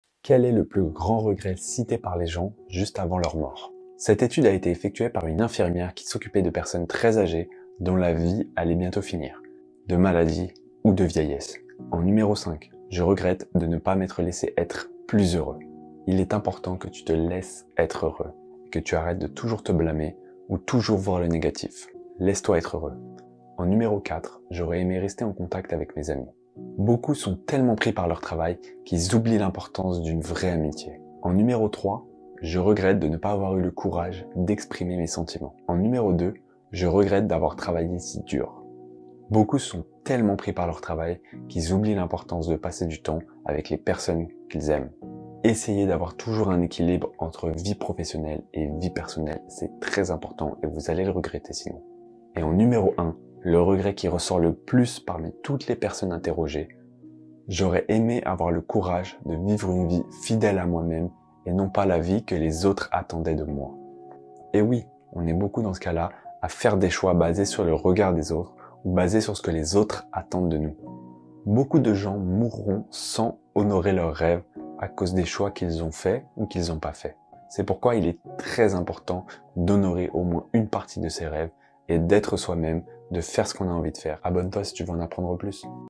Les 5 Regrets les Plus Cités par les Mourants - À voir absolument !
Ce sont les 5 regrets les plus cités par les gens, juste avant leur mort.
N'hésitez pas à me faire des retours dans les commentaires et mettre un like si vous avez aimé :)